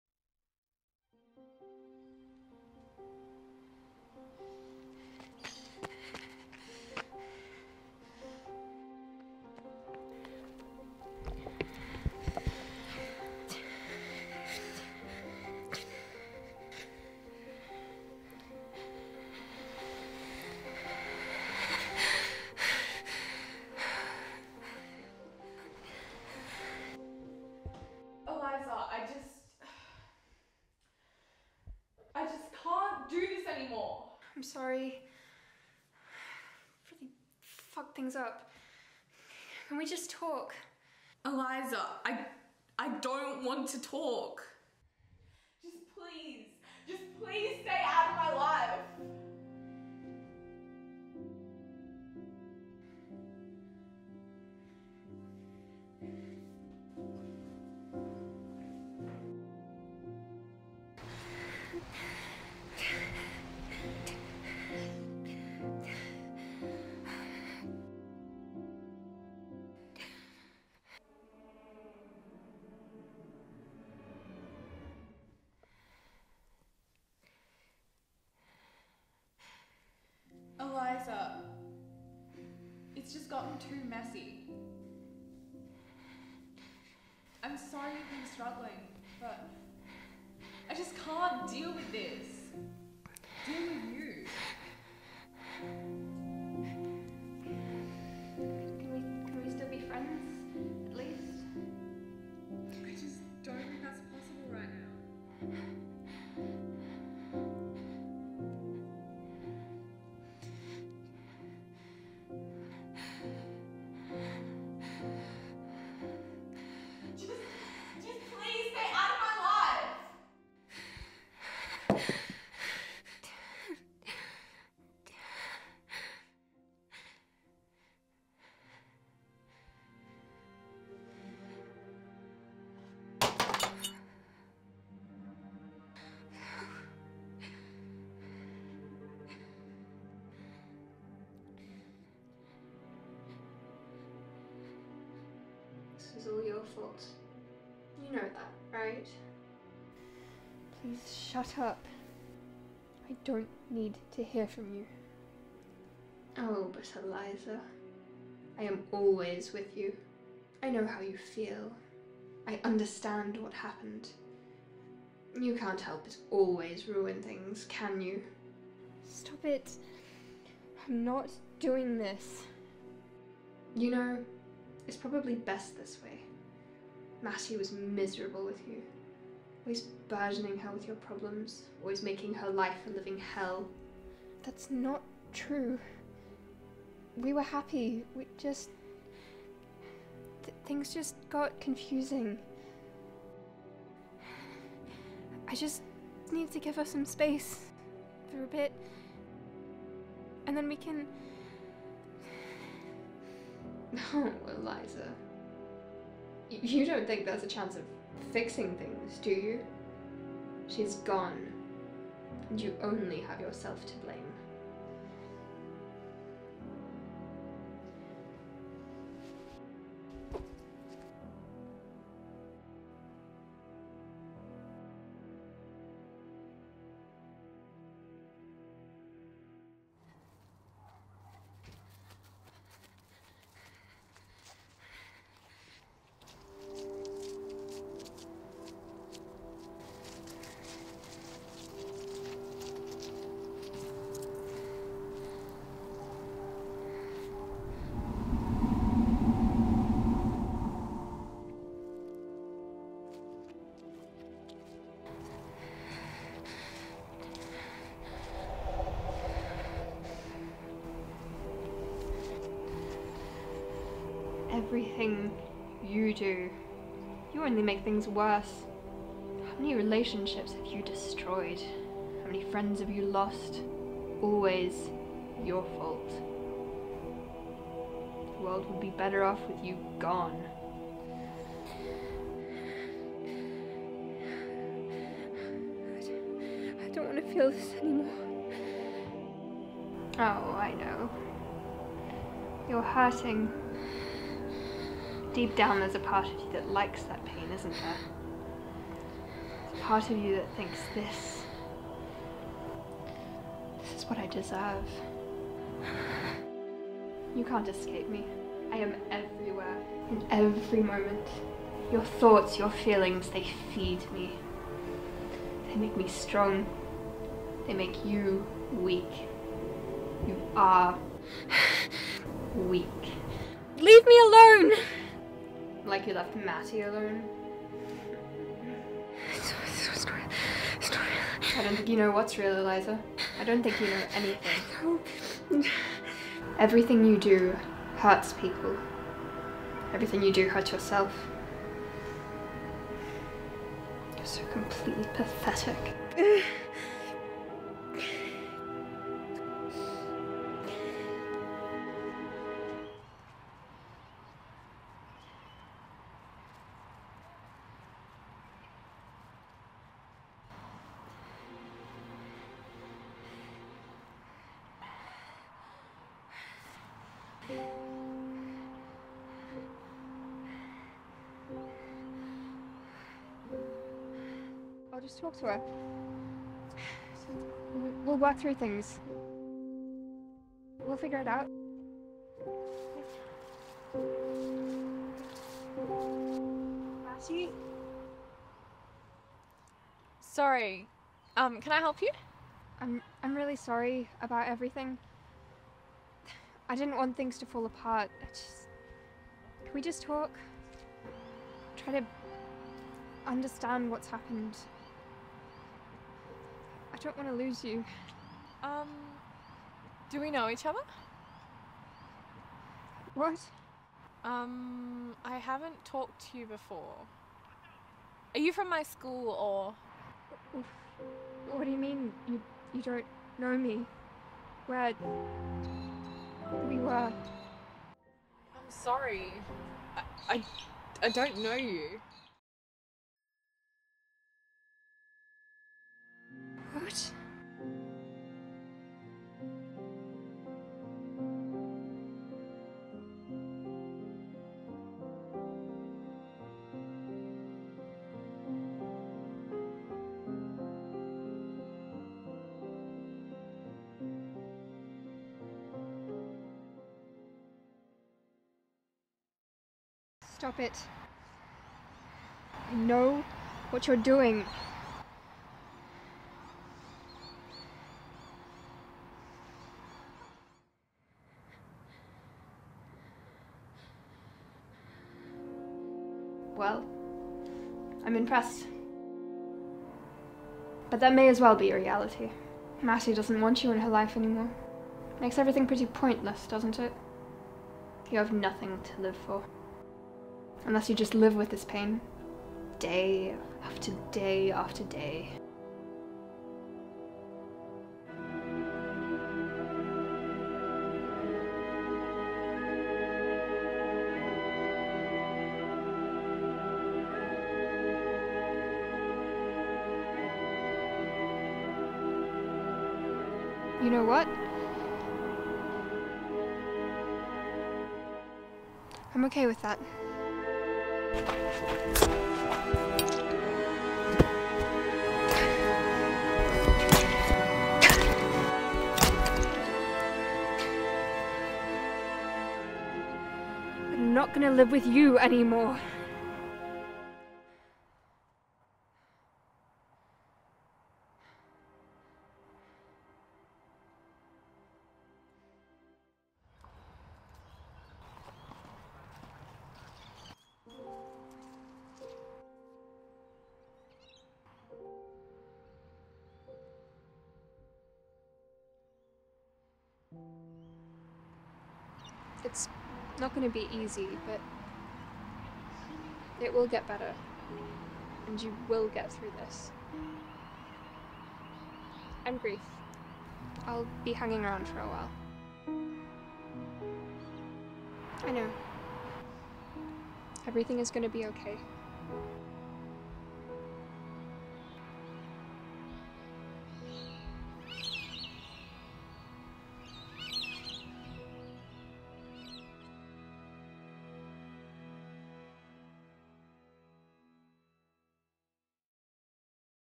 BROKEN - Year 12 Media film
Content Warning: Depictions of heartbreak, mental health struggles and suicidal ideation. Some possibly frightening scenes for younger viewers.